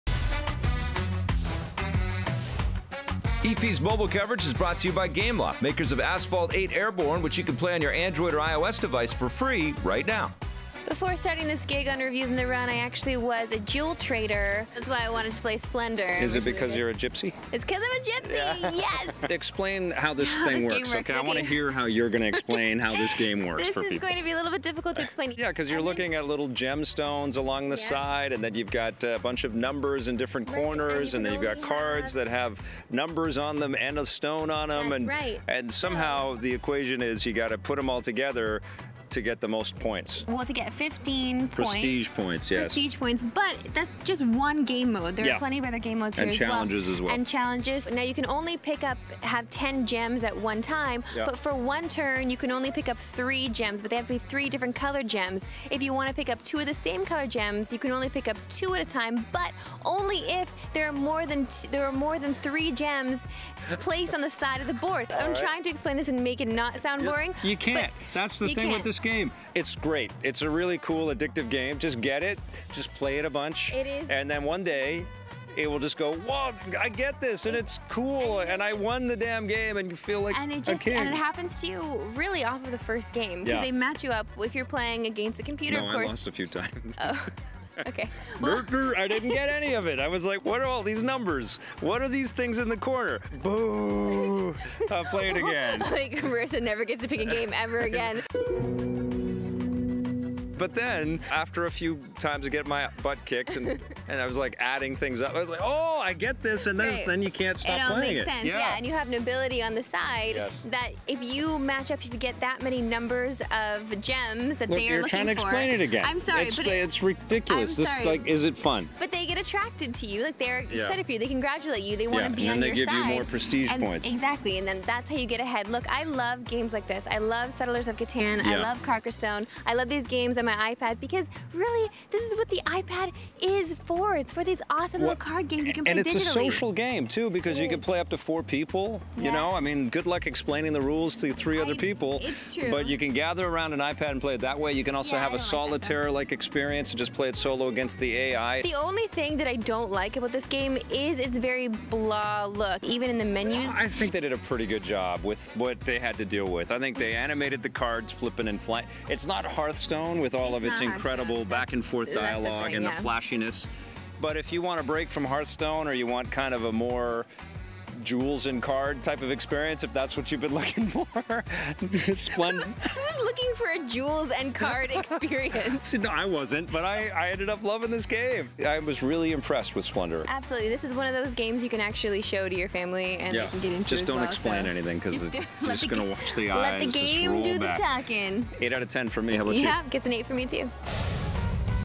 Splendor Game Review
Vic and Marissa let us know just how splendid the new digital card battle game Splendor really is.